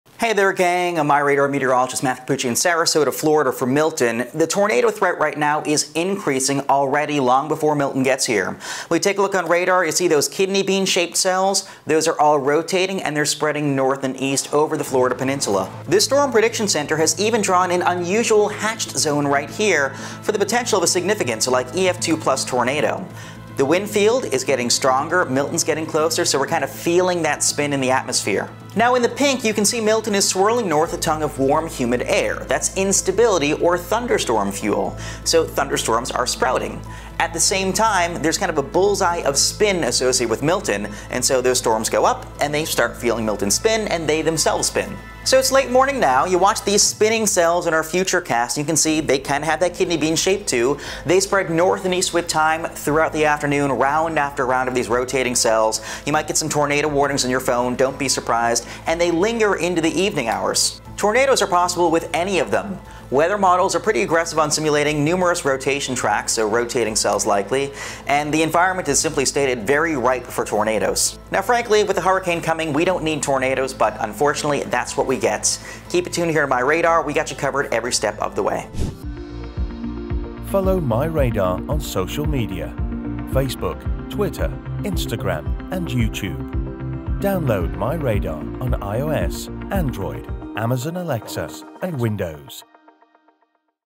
Tornado risk increasing
The risk of tornadoes is growing in central and southern portions of the Florida Peninsula and Keys. An isolated significant (EF2+) tornado is possible. The risk increases quickly through noon along the Gulf Coast, then spreads inland.

Tap for update from Matthew Cappucci.